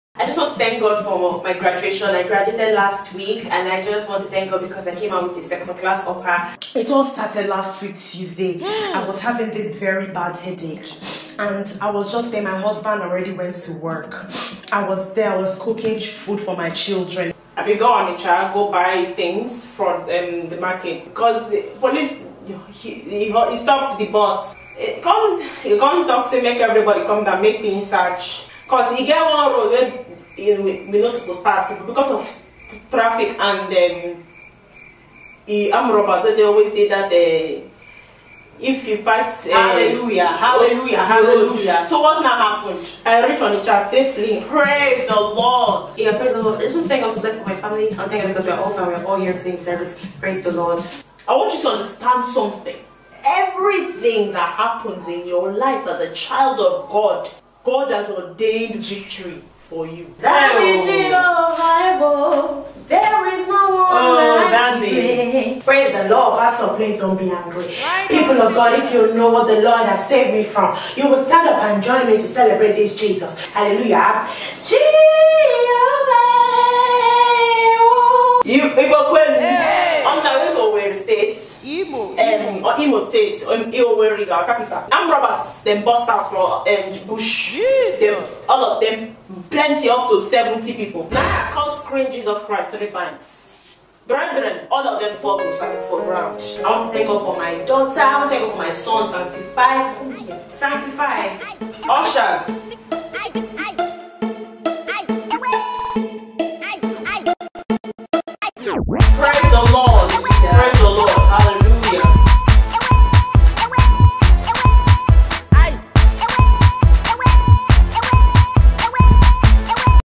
Different types of Testifiers in Church: Funny 🤣🤣video from Miraji's World (Nigeria)
Funny and interesting comedy from Miraji's World